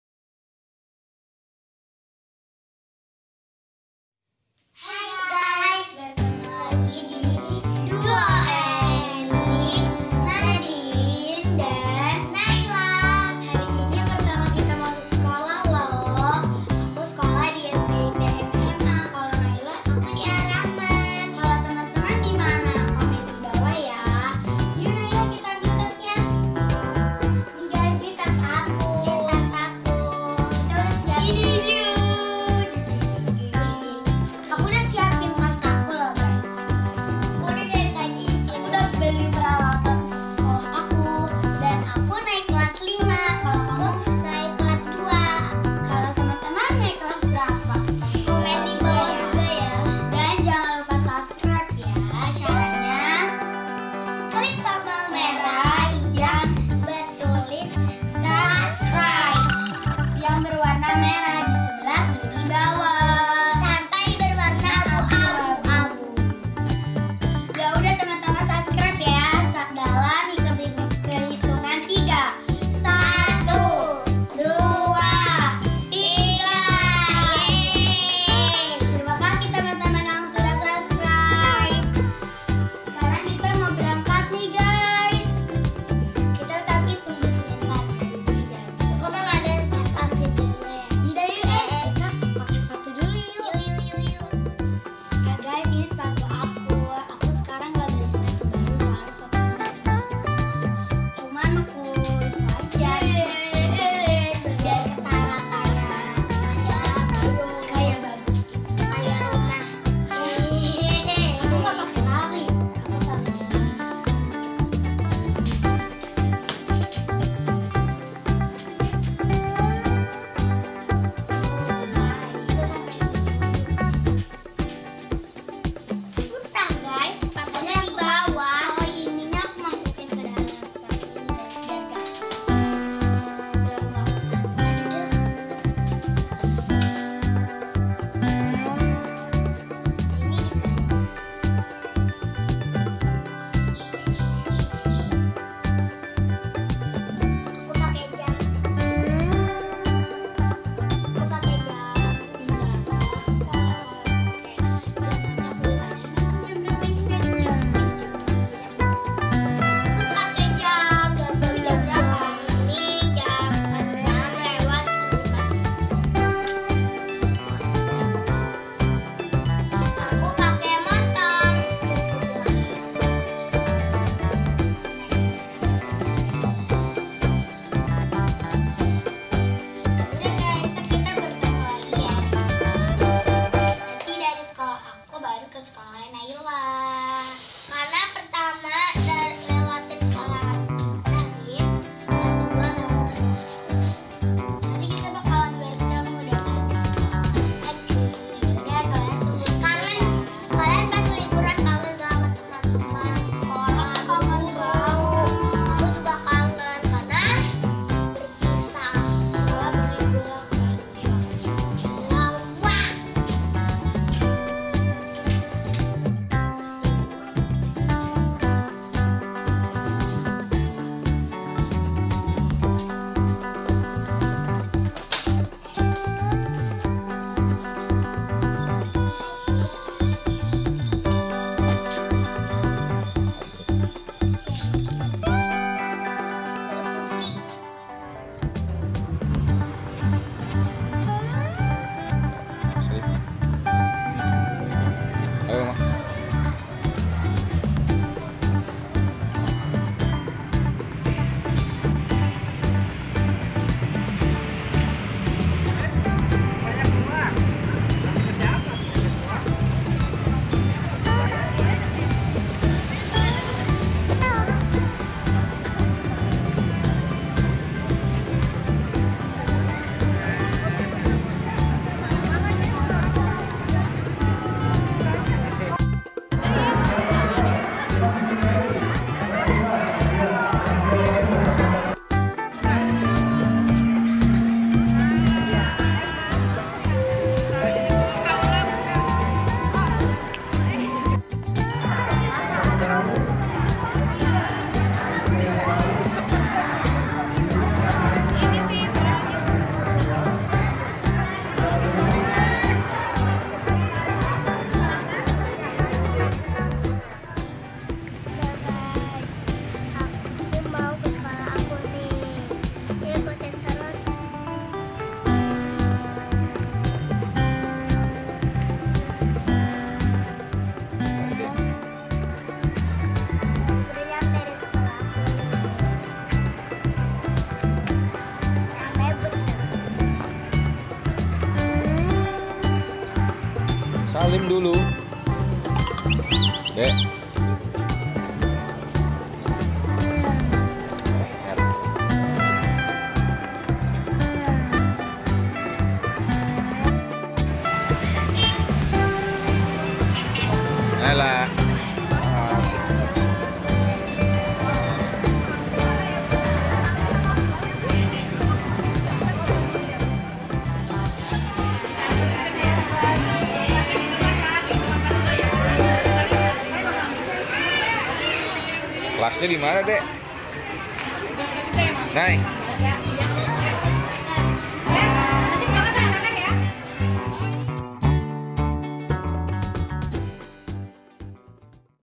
KEMBALI BERSEKOLAH - BACK TO SCHOOL | NADINE NAILA
Setelah libur sekolah yang sangat panjang, Nadine dan Naila kembali masuk ke sekolah. Nadine yang bersekolah di SDIT Mitra Mendidik Anak Atau MMA sekarang telah kelas 5 SD dan Naila sekarang telah kelas 2 SD Di SDIT Ar Rahman.
Nadine dan Naila sangat senang kembali bersekolah bertemh dengan teman-teman dan ibu guru. Kalau kamu bagimana, senang tidak kembali ke sekolah ?